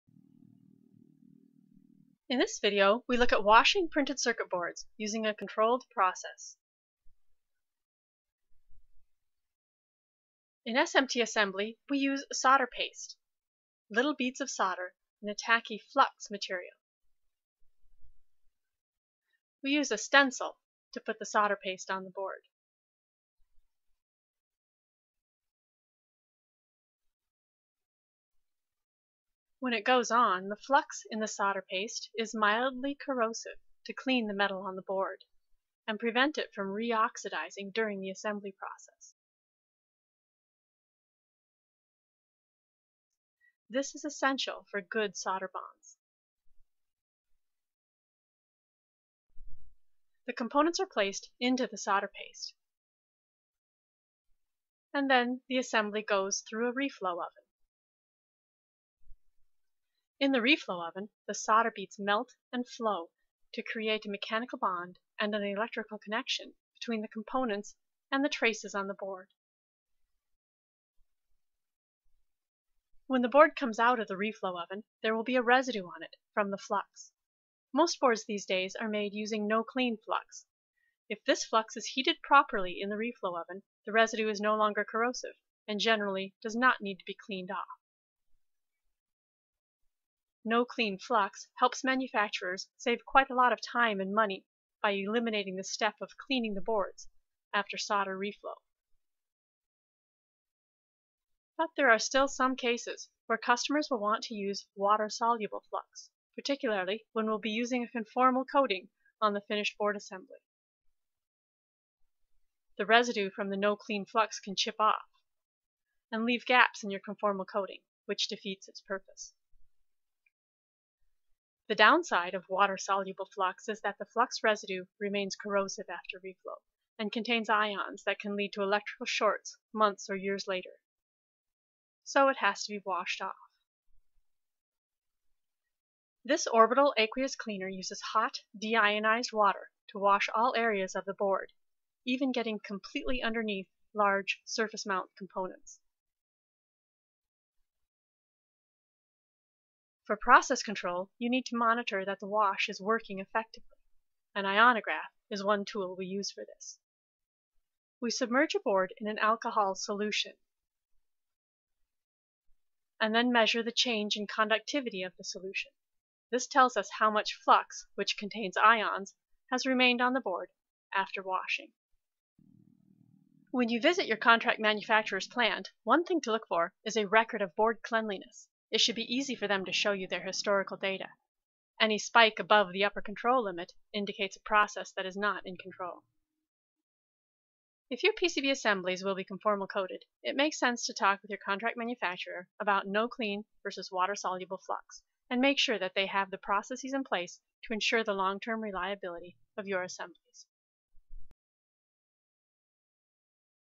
Washing PCB assemblies using a controlled process
If your PCB assemblies will be conformal coated, you should ask your contract manufacturer about no-clean vs. water-soluble flux, and their controlled processes for board cleaning to ensure long-term reliability.